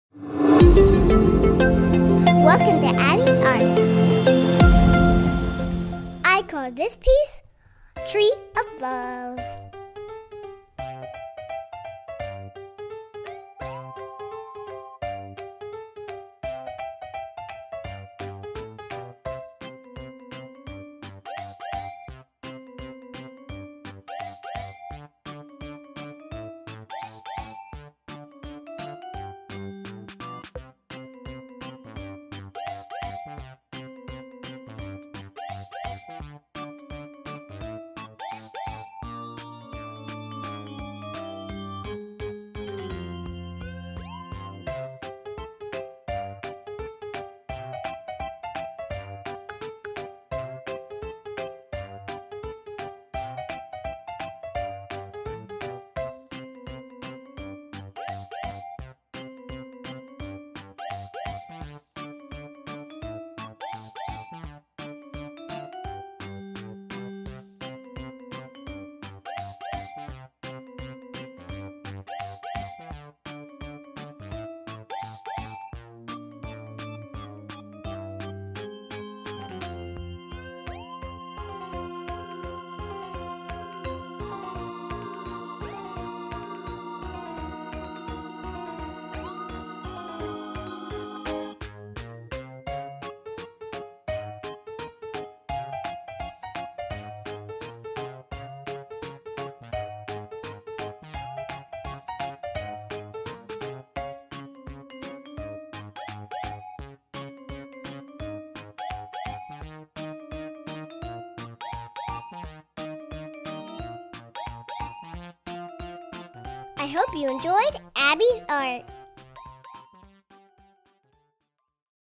Abby’s Art #2 - Tree Of Love
Welcome back to Abby's Art Channel! In Episode 2, Abby creates a stunning "Tree of Love" that celebrates the beauty of nature and the power of love. Watch as our creative young artist takes you through each step of this heartwarming project, spreading positivity and joy. 🎨❤️